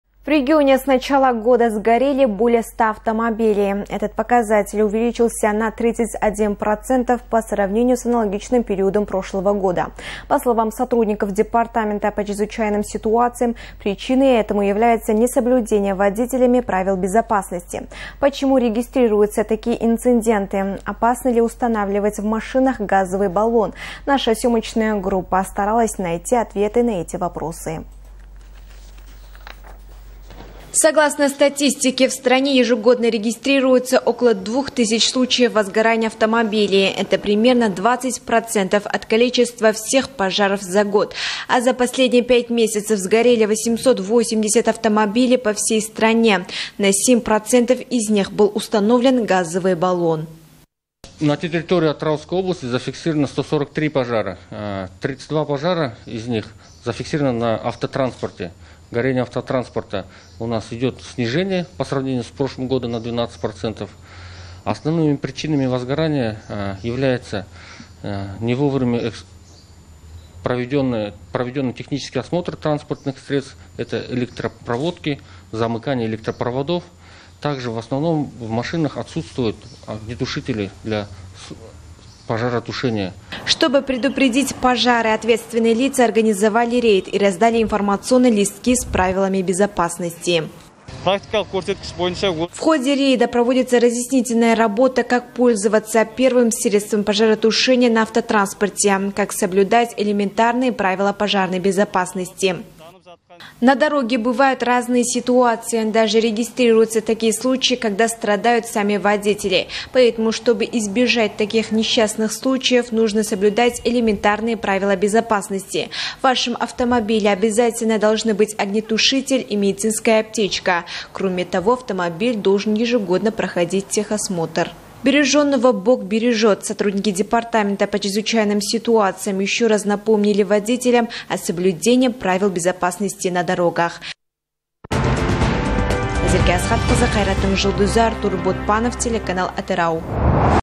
Растёт количество пожаров на автотранспорте
В регионе с начала года сгорели более 100 автомобилей.
Этот показатель увеличился на 31% по сравнению с аналогичным периодом прошлого года. По словам сотрудников департамента по чрезвычайным  ситуациям , причиной этому является несоблюдение водителями правил безопасности. Почему регистрируются такие инциденты? Опасно ли устанавливать в машинах  газовый баллон? Наша съемочная группа старалась найти ответы на эти вопросы.